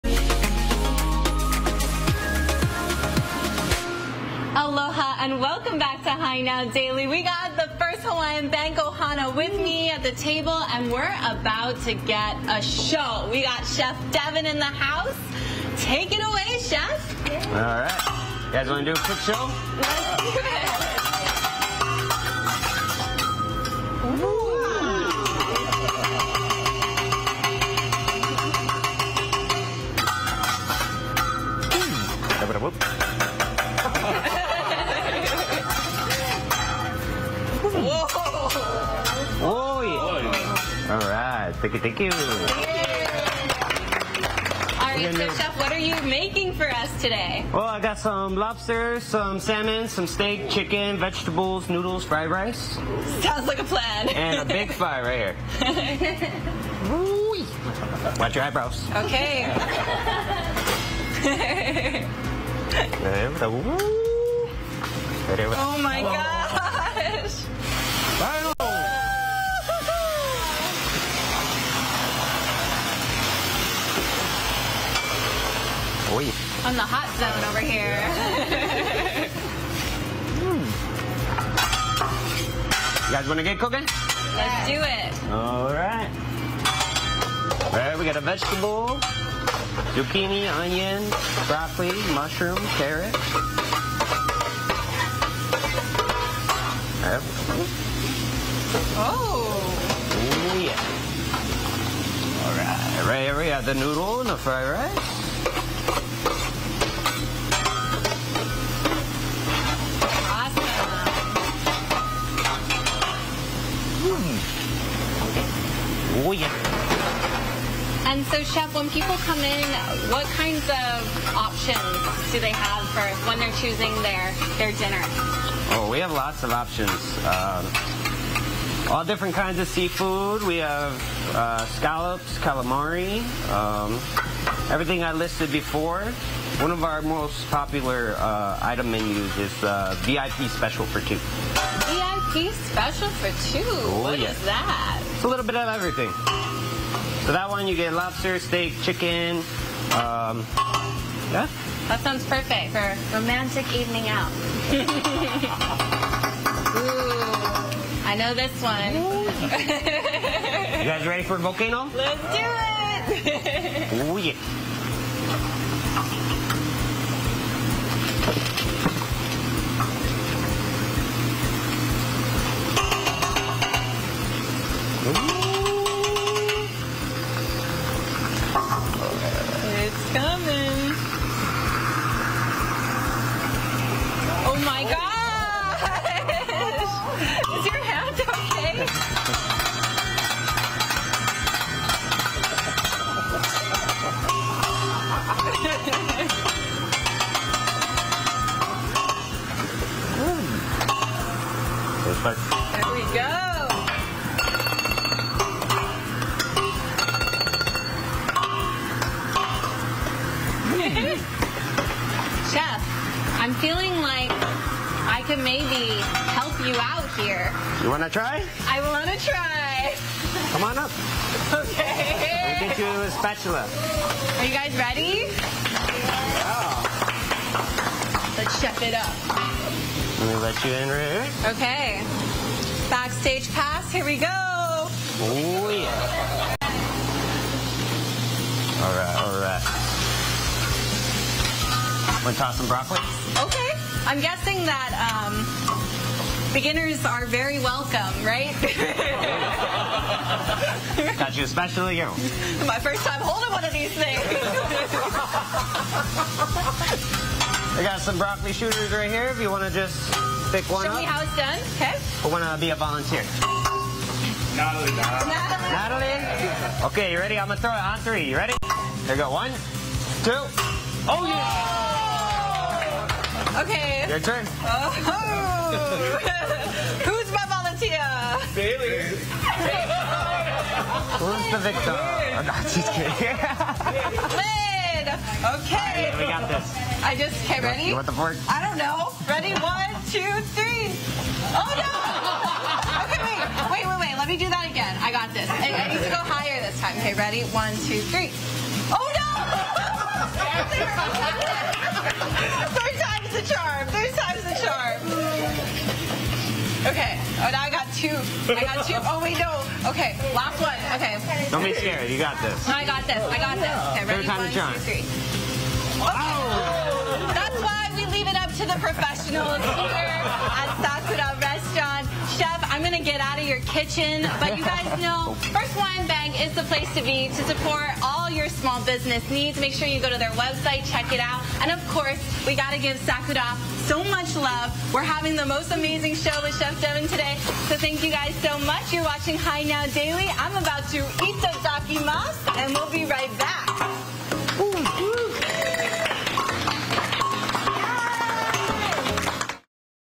Open for Business Presented by First Hawaiian Bank: Sakura (Part 3)
First Hawaiian Bank celebrates Small Business Month in a big way! HI Now gets a preview of what the Bank is doing with partners like Sakura Japanese Restaurant!